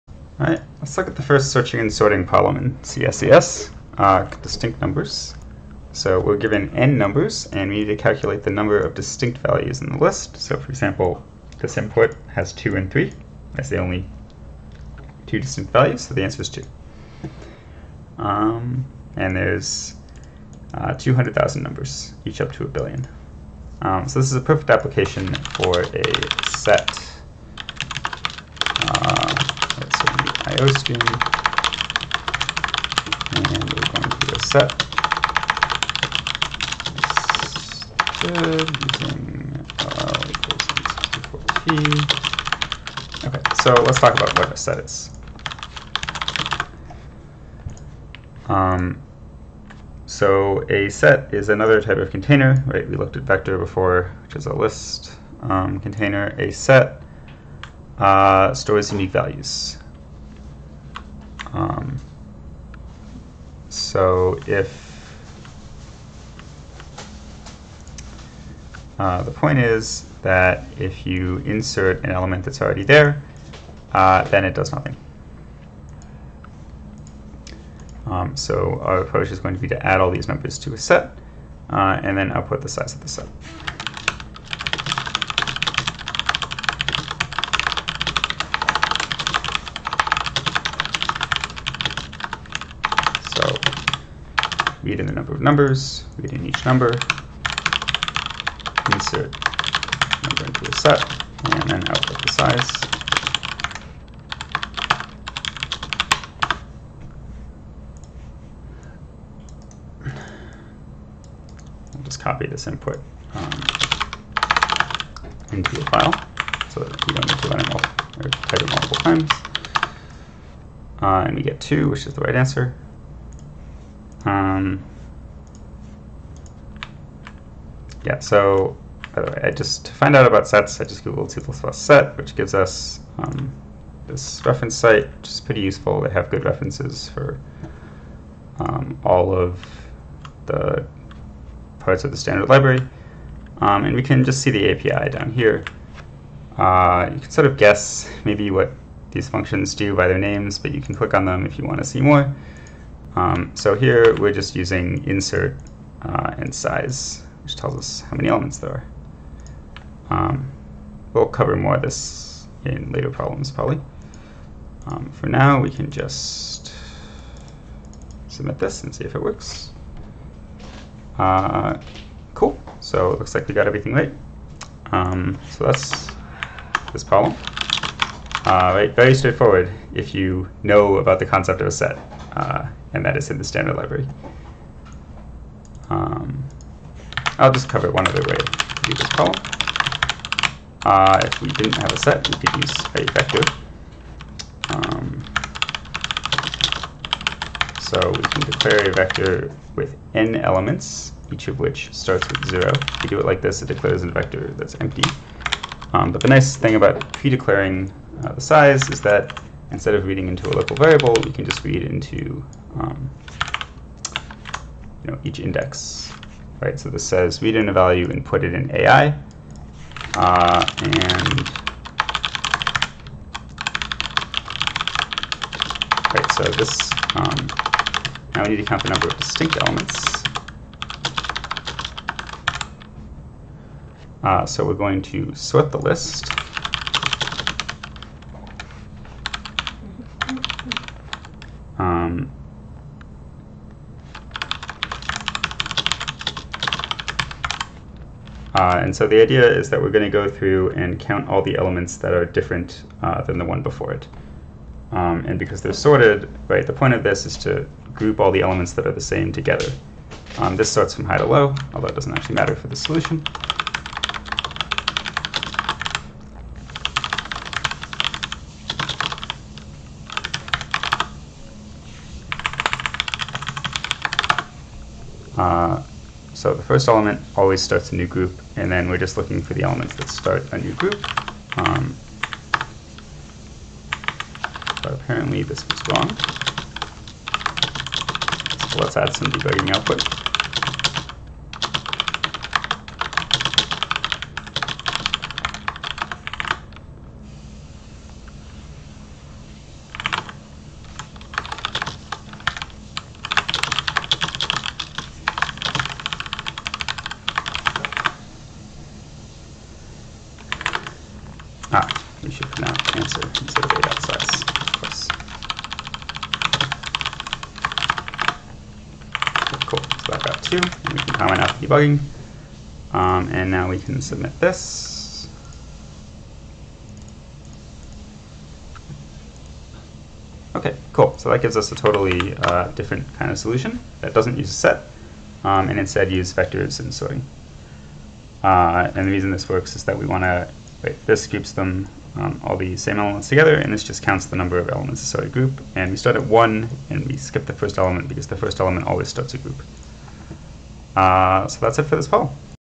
Competitive Programming Guide - Searching and Sorting #1
I didn't discuss running time during the video, so let me do it here. Because N  is up to 200,000, an O(N^2) solution would be too slow. The two solutions I showed in the video are both O(N log N).

An example of a solution that would be too slow is a "manually implemented" set, where you store a vector of "distinct numbers so far", and compare each element to each of the "distinct numbers so far" every time. In the worst case where all the numbers are distinct, you would end up comparing all pairs of numbers which is 20 billion comparisons, which is too slow.